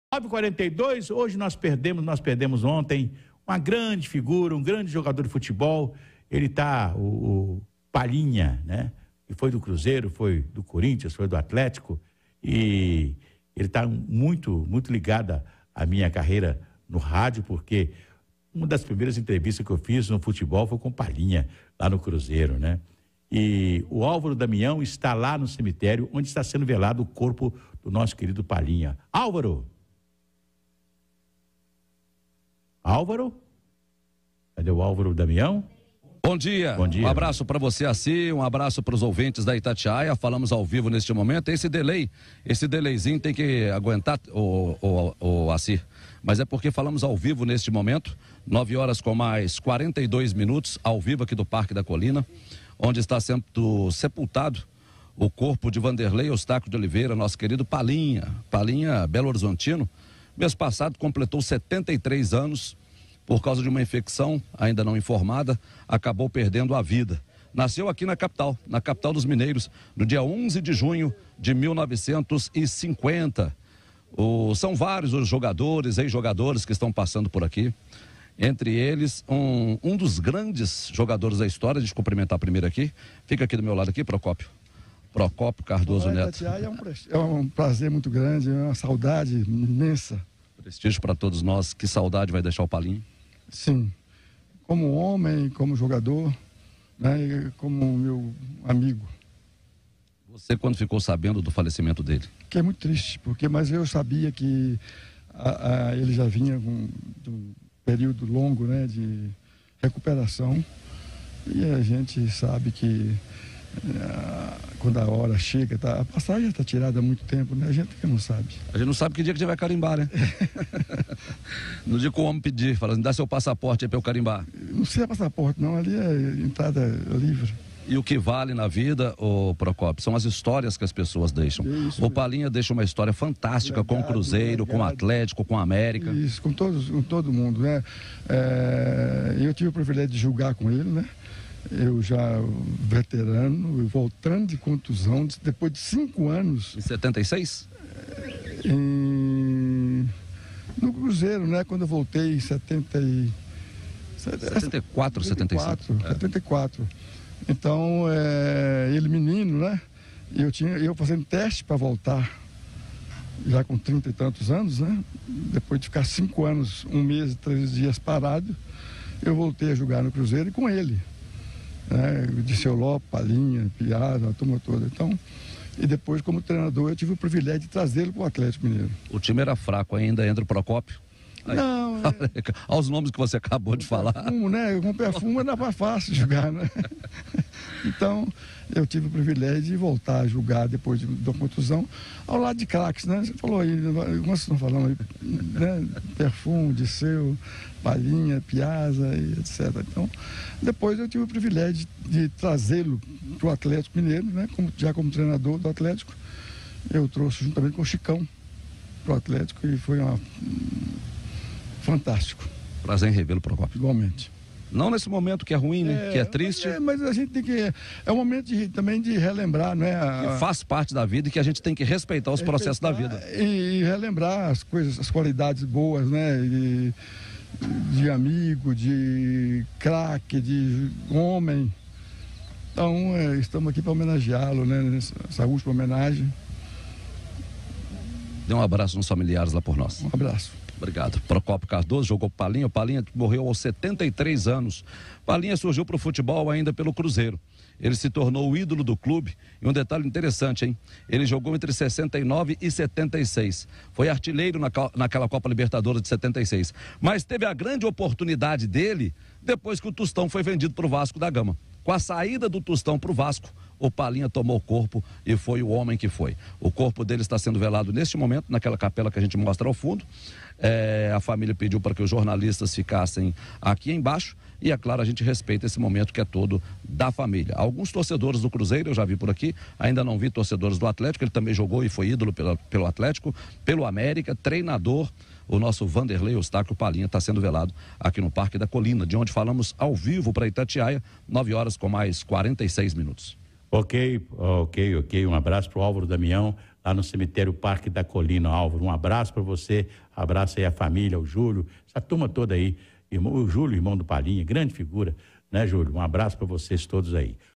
PROCÓPIO CARDOZO COMENTA SOBRE SUAS HISTÓRIAS COM O PALINHA DIRETO DO CEMITÉRIO DO PARQUE DA COLINA
Álvaro Damião fala direto do cemitério Parque da Colina, onde está sendo velado o ex-jogador Palinha.